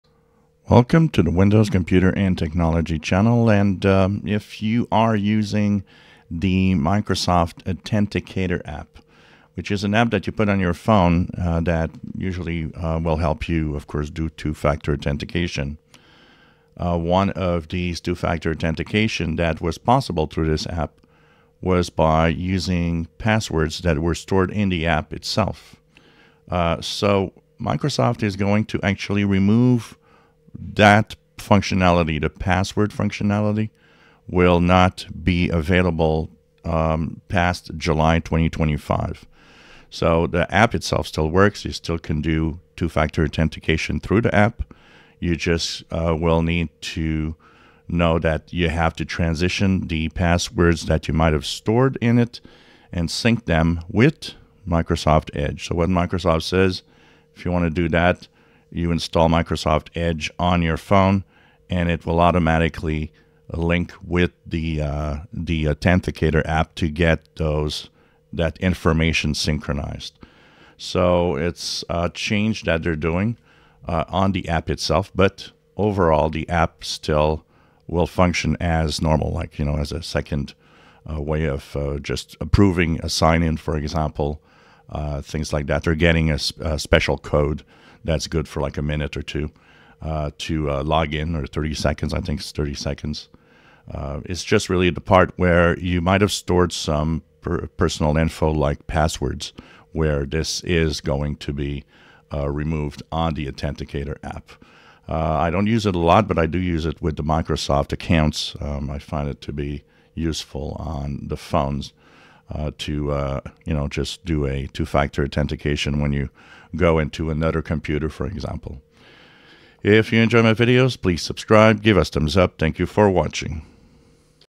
Login changes coming to the Microsoft Authenticator app moves to EDGE
Saved passwords and credentials will move to EDGE from Authenticator app